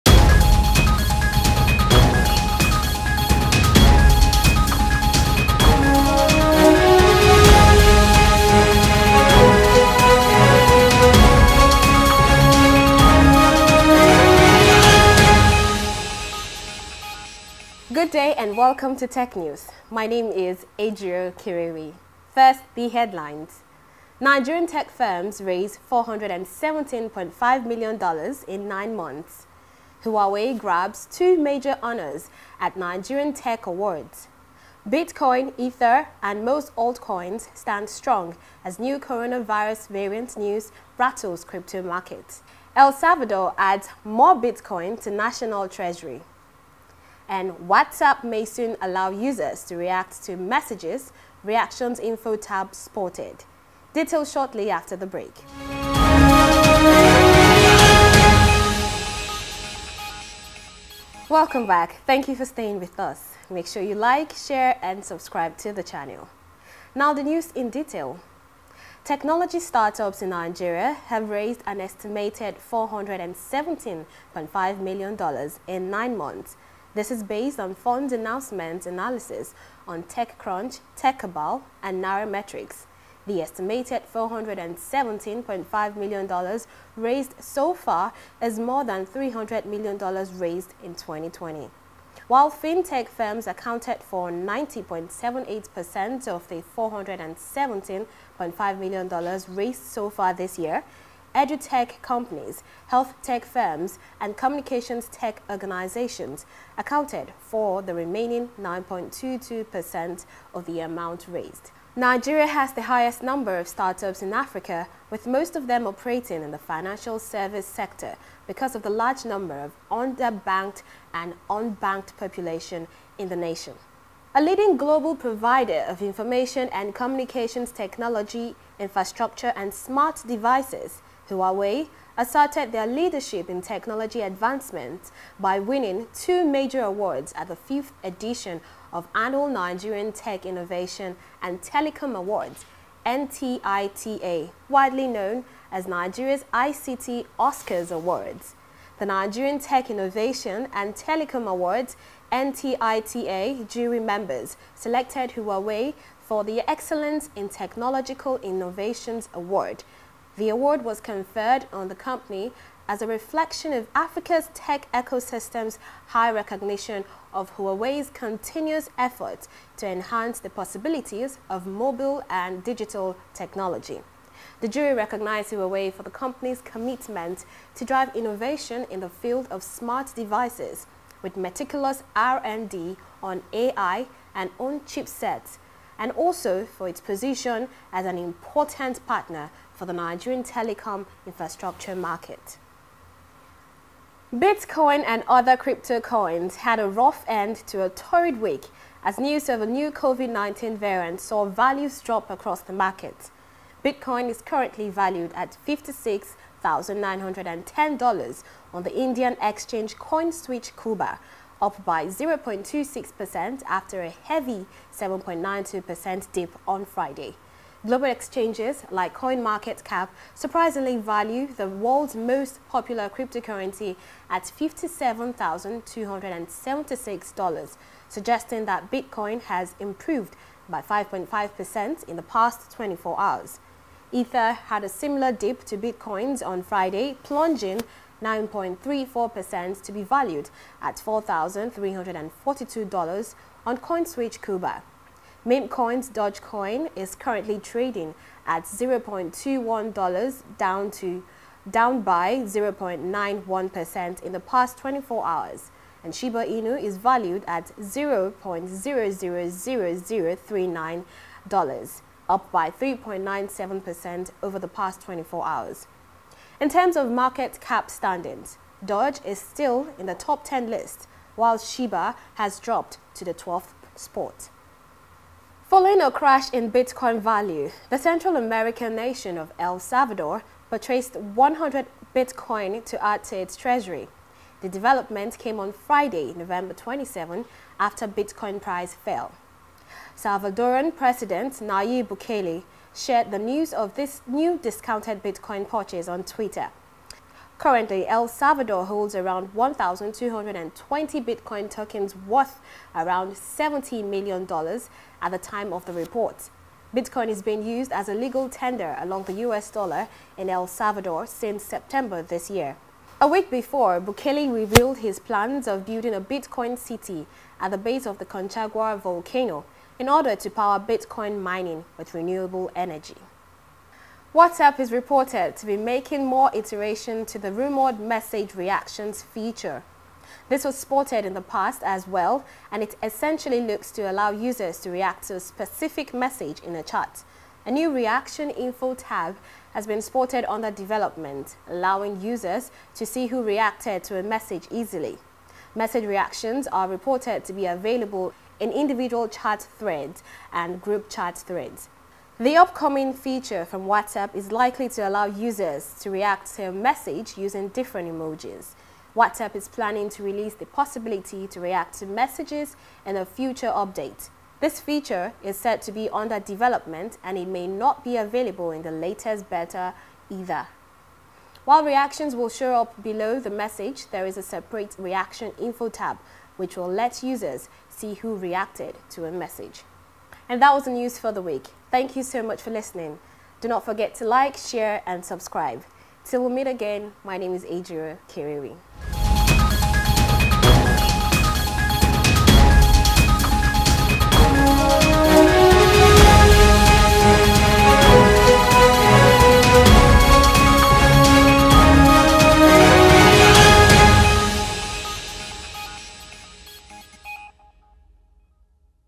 NIGERIAN TECH FIRMS RAISES $417,000,000 in 9 MONTHS
Bringing you the highlights of the events in the week , across the technology industry.